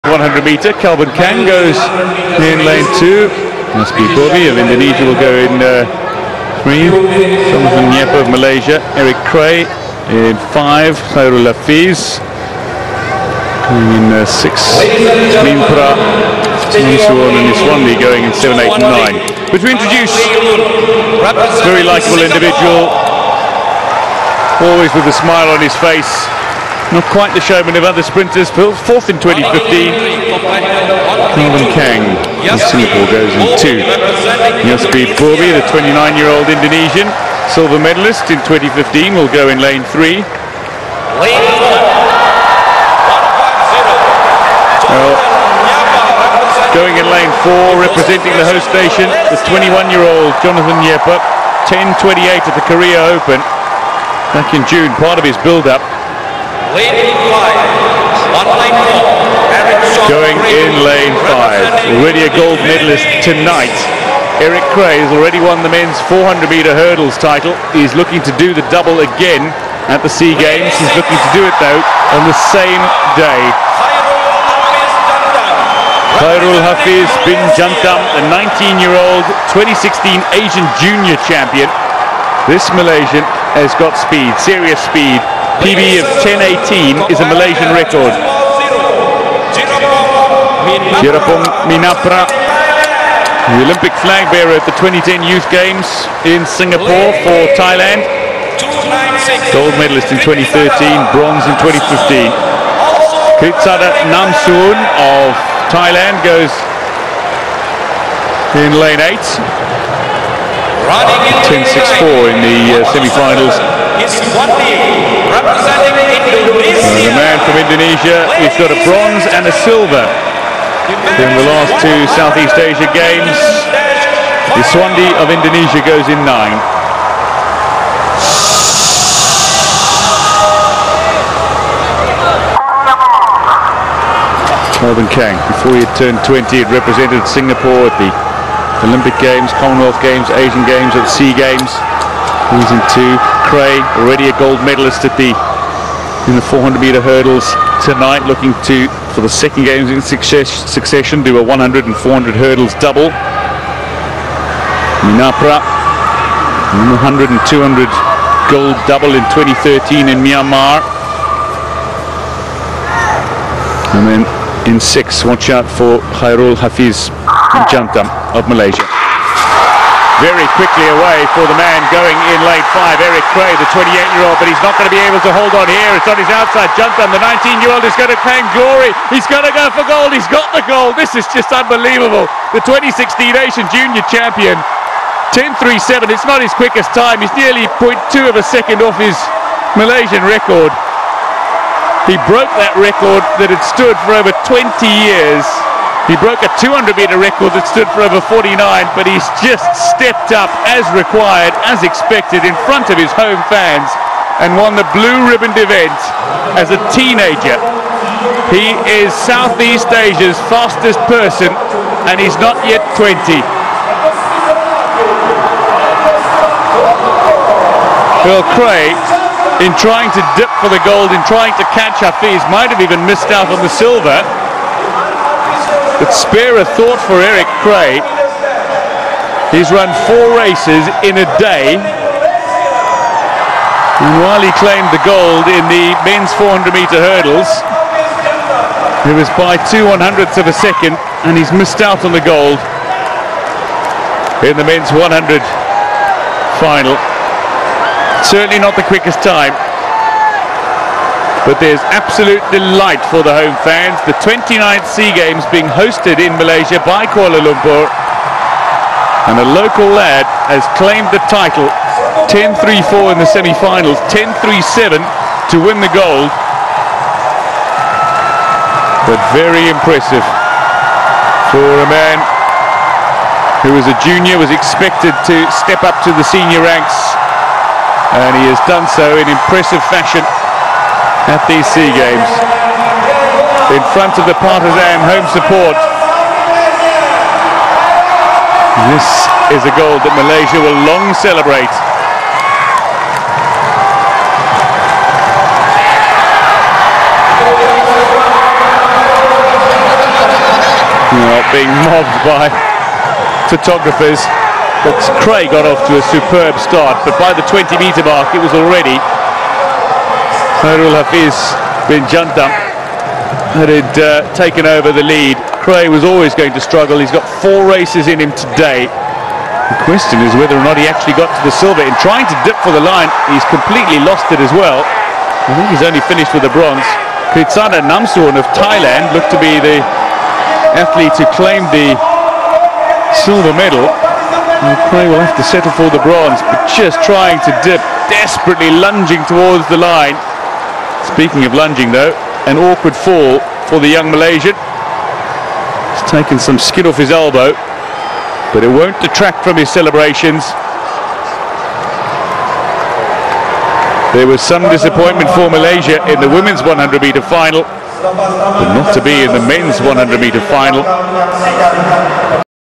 Athletics Men's 100m Finals | 29th SEA Games 2017
Athletics Men's 100m Finals of the 29th SEA Games 2017 on 22 August 2017

Gold - Khairul Hafiz B J (MAS) 10.38
Silver - Eric Shauwn Cray (PHI) 10.43
Silver - Kritsada Namsuwun (THA) 10.43

Follow the action on OneTeamSG

❤ """"COMMENTS"""" below on your favourite moment from the match.